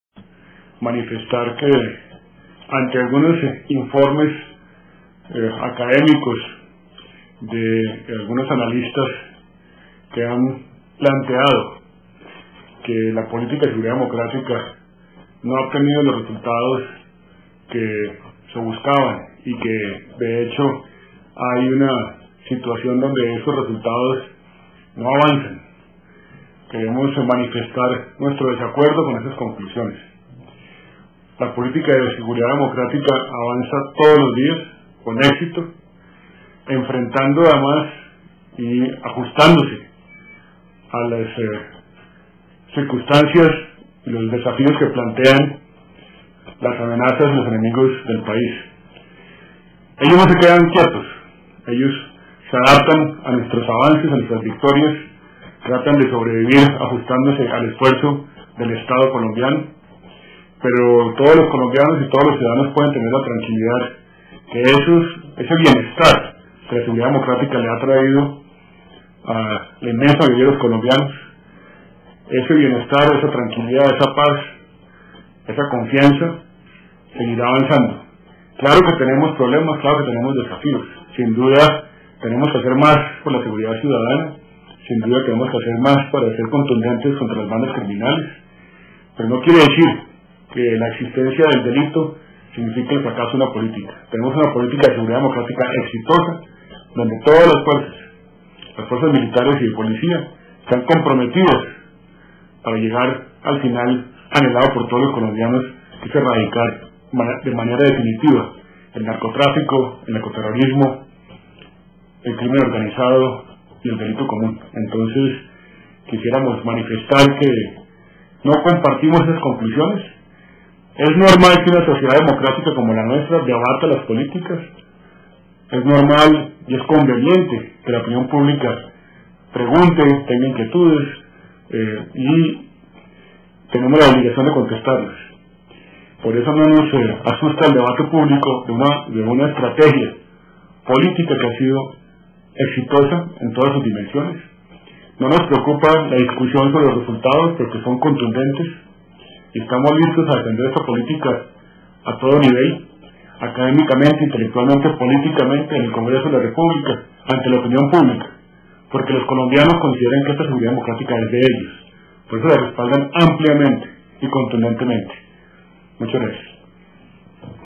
Seguridad Democrática seguirá beneficiando a los colombianos, sostiene Mindefensa
26 de noviembre de 2009 - El ministro de defensa, Gabriel Silva, destacó los avances de la Política de Seguridad Democrática en su propósito de devolverles la paz a los colombianos, y expresó el desacuerdo del Gobierno con el informe sobre la misma presentado ayer por una Organización No Gubernamental. También resaltó la posibilidad de que las políticas puedan ser debatidas a la luz pública en el marco de la democracia colombiana.